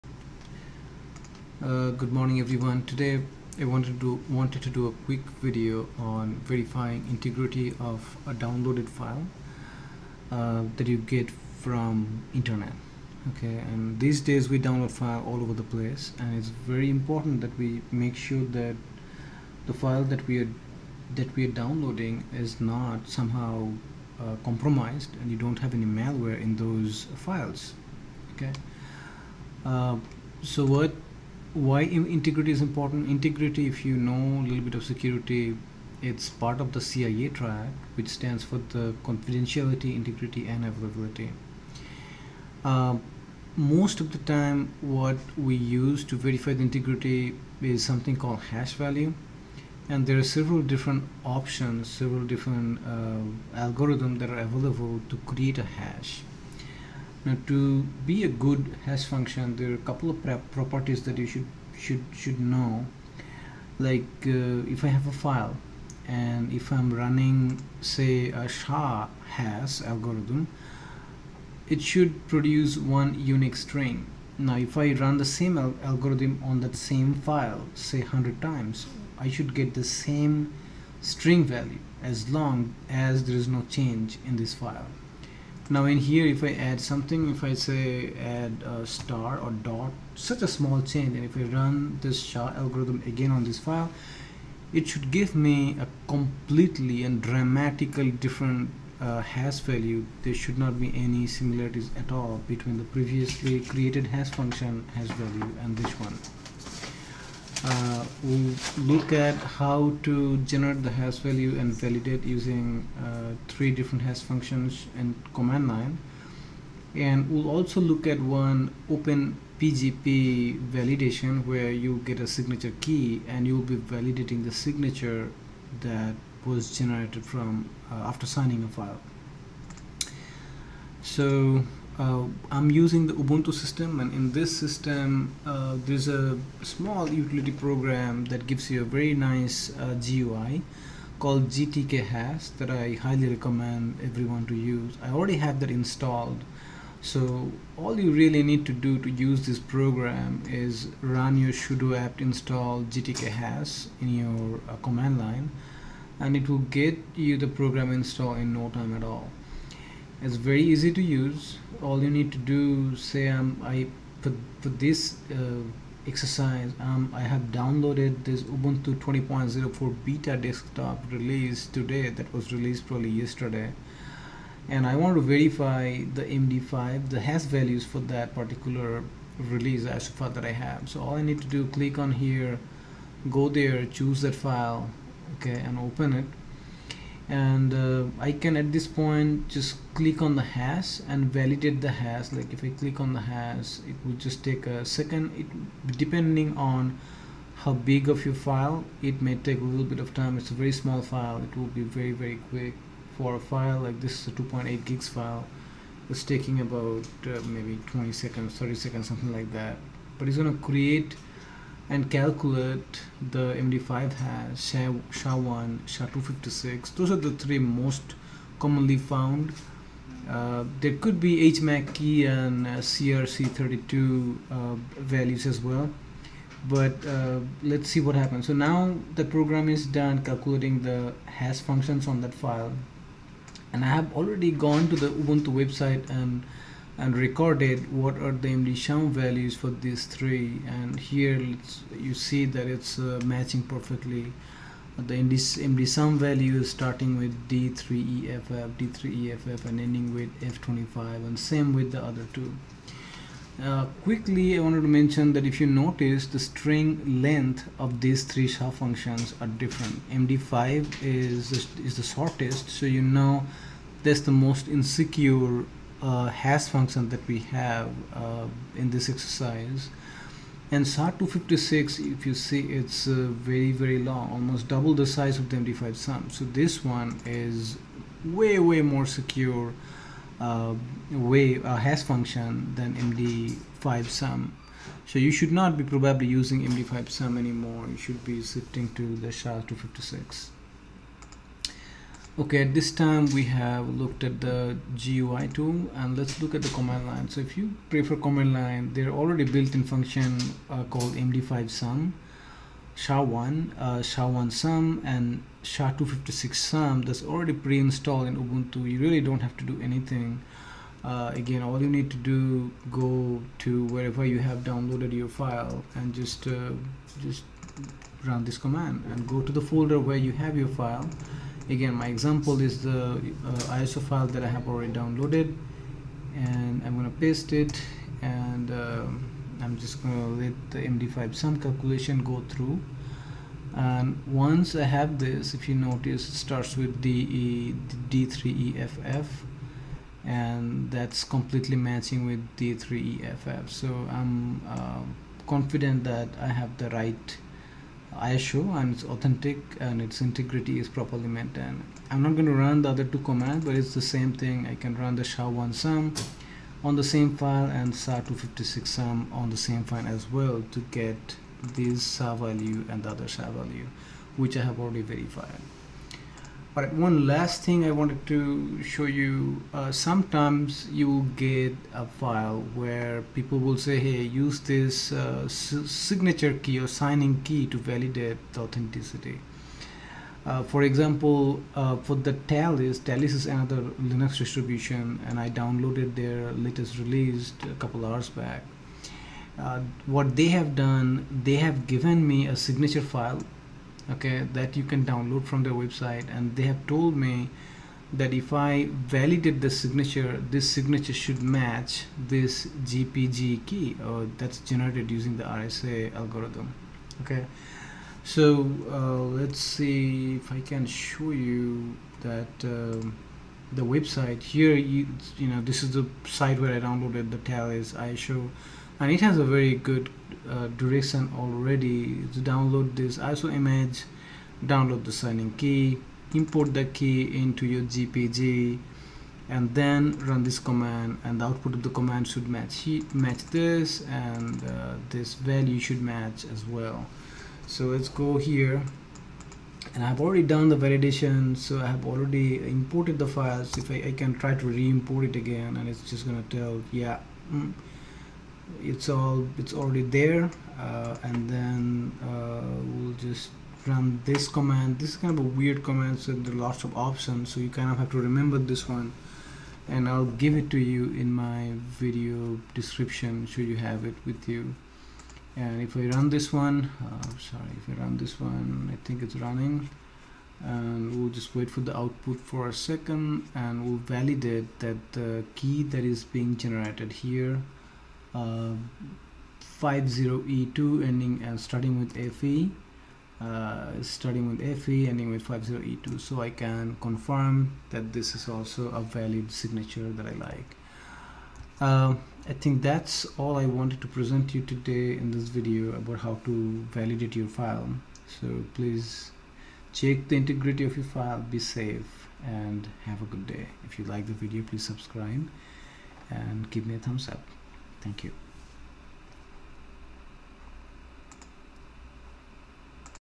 File Integrity Check in Ubuntu 19.10 Linux - SHA1, SHA256, MD5, Open-PGP
Verify File integrity using hash - SHA1, SHA256, MD5, OpenPGP.

CIA Triad – Confidentiality, Integrity and Availability.
We download from many sources.
Learn how to verify integrity by cross checking hash value of a file

Hash – one way function.

GUI - sudo apt install gtkhash
Command-line - use MD5SUM, SHA1SUM or SHA256SUM

OpenPGP, import signature key to gpg.